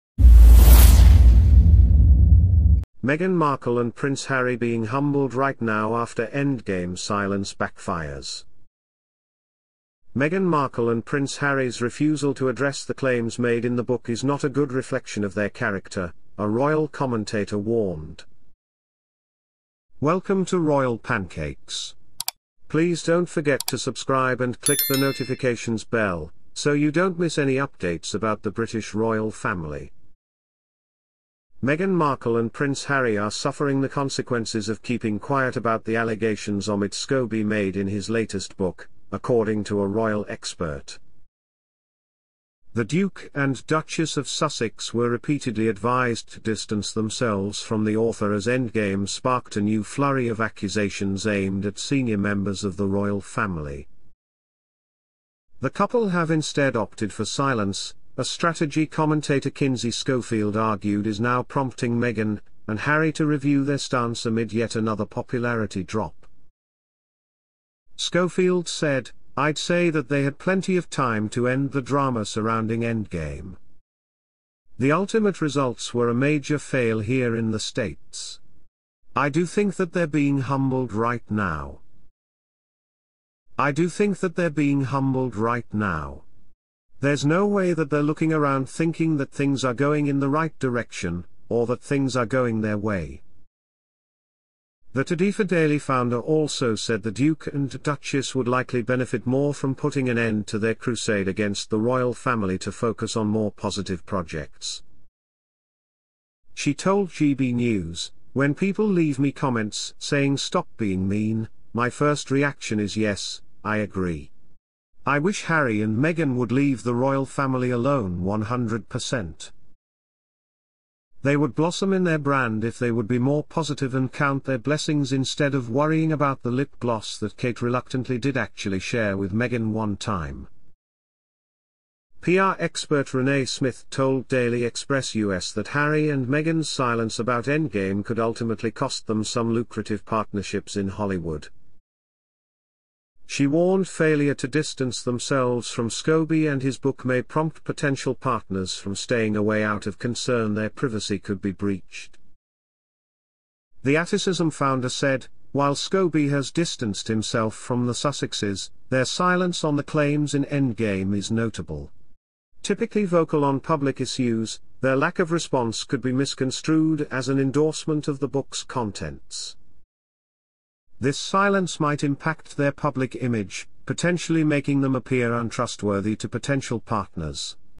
FORGIVE US!🛑 Meghan & Harry kneel to King Charles for forgiveness after Endgame attacks backfire
Meghan Markle and Prince Harry 'being humbled right now' after Endgame silence backfires.

Meghan Markle and Prince Harry's refusal to address the claims made in the book is 'not a good reflection of their character,' a royal commentator warned.

Meghan Markle and Prince Harry are suffering the consequences of keeping quiet about the allegations Omid Scobie made in his latest book, according to a royal expert.

The Duke and Duchess of Sussex were repeatedly advised to distance themselves from the author as Endgame sparked a new flurry of accusations aimed at senior members of the Royal Family.

The couple have instead opted for silence, a strategy commentator Kinsey Schofield argued is now prompting Meghan and Harry to review their stance amid yet another popularity drop.

Schofield said: "I'd say that they had plenty of time to end the drama surrounding Endgame.
"The ultimate results were a major fail here in the States. I do think that they're being humbled right now.

"I do think that they're being humbled right now. There's no way that they're looking around thinking that things are going in the right direction, or that things are going their way."

The To Di For Daily founder also said the Duke and Duchess would likely benefit more from putting an end to their crusade against the Royal Family to focus on more "positive" projects.

She told GB News: "When people leave me comments saying stop being mean, my first reaction is yes, I agree. "I wish Harry and Meghan would leave the Royal Family alone 100 percent.

"They would blossom in their brand if they would be more positive and count their blessings instead of worrying about the lip gloss that Kate reluctantly did actually share with Meghan one time."

PR expert Renae Smith told Daily Express US that Harry and Meghan's silence about Endgame could ultimately cost them some lucrative partnerships in Hollywood.

She warned failure to distance themselves from Scobie and his book may prompt potential partners from staying away out of concern their privacy could be breached.

The Atticism founder said: "While Scobie has distanced himself from the Sussexes, their silence on the claims in "Endgame" is notable. "Typically vocal on public issues, their lack of response could be misconstrued as an endorsement of the book's contents.

"This silence might impact their public image, potentially making them appear untrustworthy to potential partners."princeharry